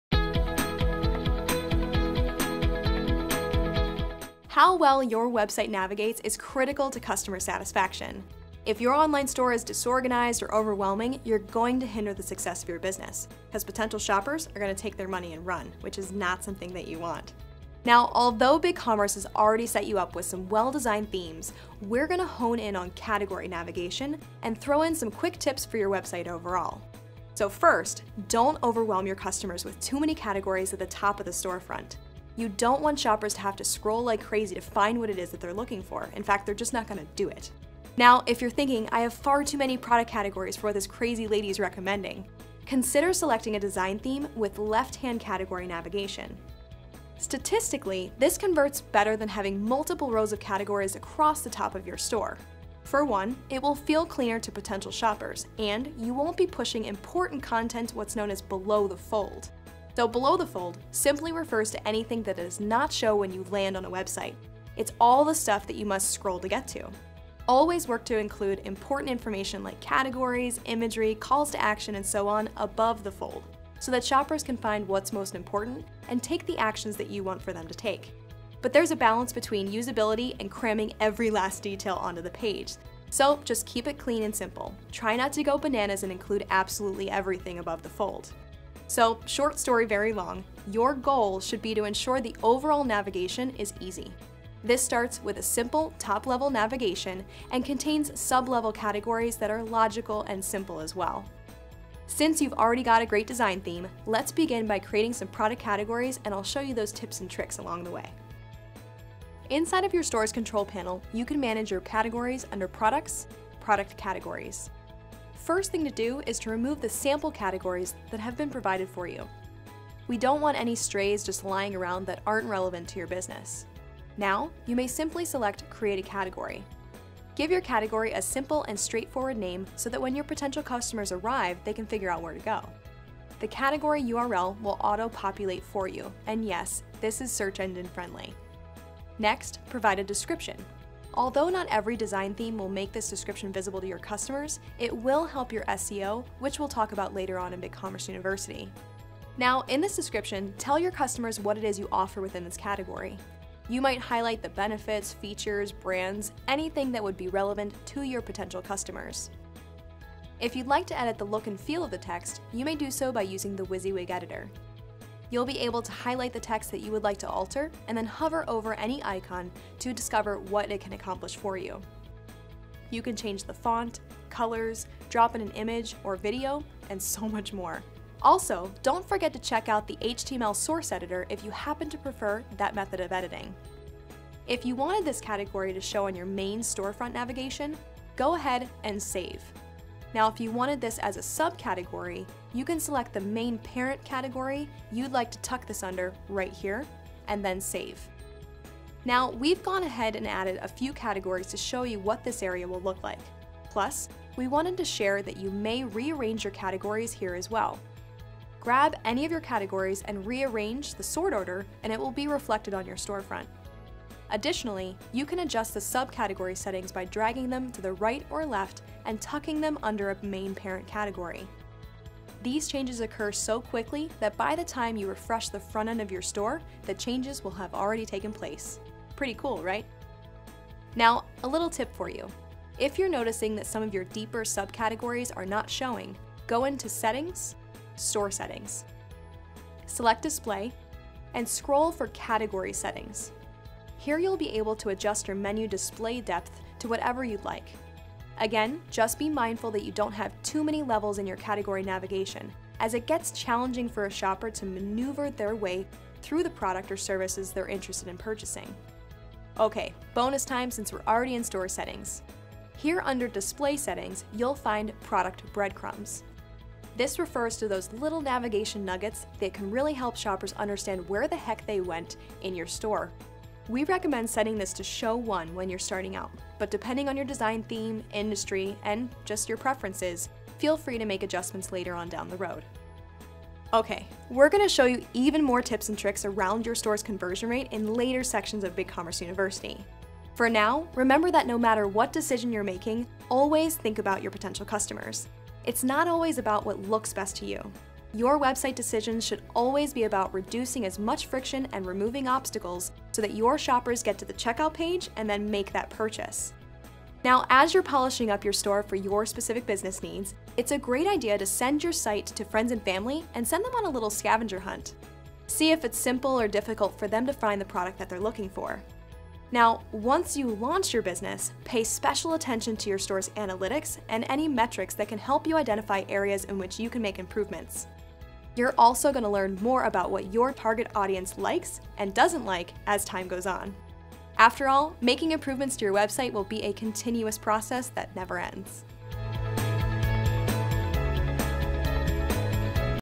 Product Categories - Launch Store | Bigcommerce University
Planning and creating your categories is key to your store's success.  Too many categories, and finding products on your store can be tough.  Too few categories, and you may end up with product-heavy category pages, making it hard for customers to find their product.  It's a fine line.  Fortunately, it's really easy here at Bigcommerce.  In this video Katey will help you plan out your categories and show you how to build and arrange them inside your Bigcommerce control panel.

Jump to...
Best practices for store categories and navigation (0:26)
Finding and deleting sample product categories (1:48)
Creating a product category (2:04)
Arranging product categories using drag n' drop (3:18)
Adjusting the display depth of your category navigation (3:50)
Selecting breadcrumb visibility (4:18)

Want advice or to ask a specific question? Reach out to our community of experts they're ready to help.